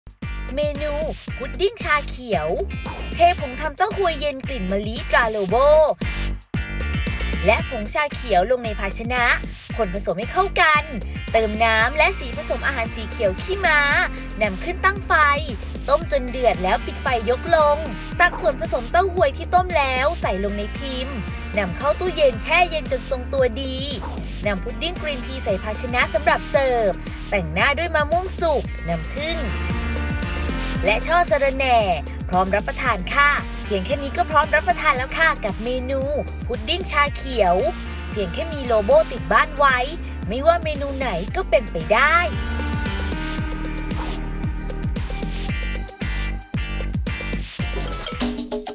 พุดดิ้งชาเขียว สูตรเด็ด ทำง่าย สไตล์ "อ.ยิ่งศักดิ์"
พุดดิ้งชาเขียว
Green tea pudding

ผลิตภัณฑ์ : ผงทําเต้าฮวยเย็นกลิ่นมะลิ ตราโลโบ

ผงทำเต้าฮวยเย็นกลิ่นมะลิ ตราโลโบ    1 ซอง
ผงชาเขียว  1 ช้อนโต๊ะ
สีผสมอาหารสีเขียวขี้ม้า  1 ช้อนชา
น้ำผึ้งสำหรับหยอดหน้า
ผลไม้สำหรับแต่ง มะม่วงสุก ตัดเป็นชิ้น
และช่อสะระแหน่สำหรับแต่ง

1.เทผงทำเต้าฮวยเย็นกลิ่นมะลิ ตราโลโบ และผงชาเขียว ลงในหม้อ คนผสมให้เข้ากัน
2.เติมน้ำและสีผสมอาหารสีเขียวขี้ม้า นำขึ้นตั้งไฟ ต้มจนเดือด ปิดไฟยกลง
3.ตักส่วนผสมเต้าฮวยที่ต้มแล้วนี้ใส่ลงในพิมพ์ จนเต็ม นำเข้าตู้เย็น แช่เย็นจนขนมทรงตัวดี
4.นำพุดดิ้งกรีนทีใส่ภาชนะสำหรับเสิร์ฟ
5.แต่งหน้าด้วยมะม่วงสุก หยอดด้วยน้ำผึ้ง และช่อสะระแหน่ พร้อมรับประทาน

▲Line @yingsakfood